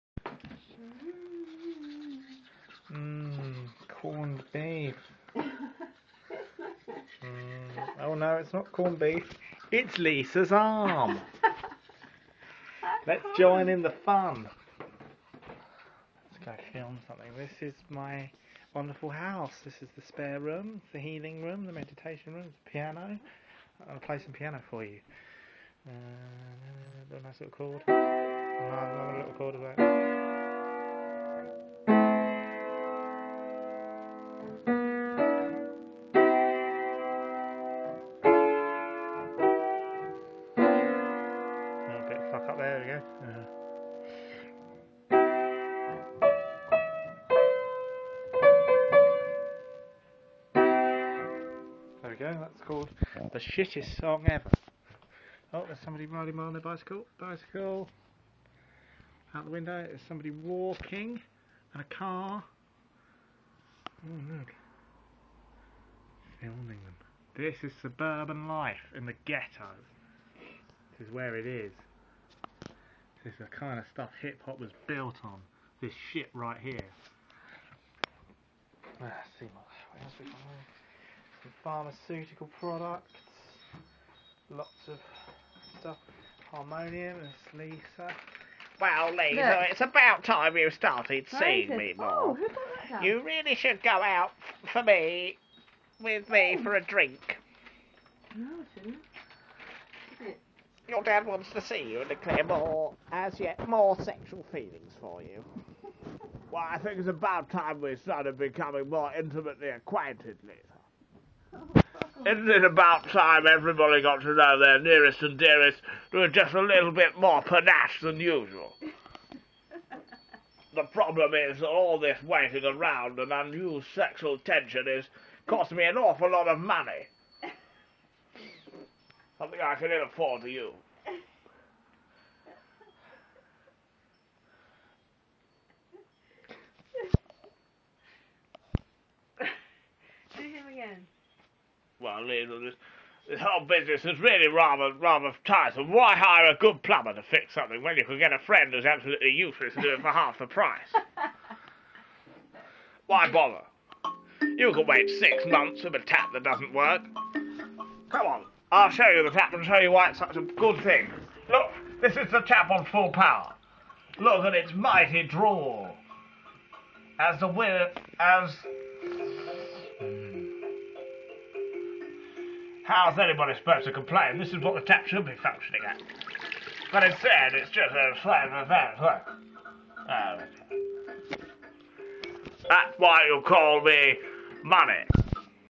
Doug's life
Shall I tell you about my life? No? Well here's a short tour of my wonderful abode...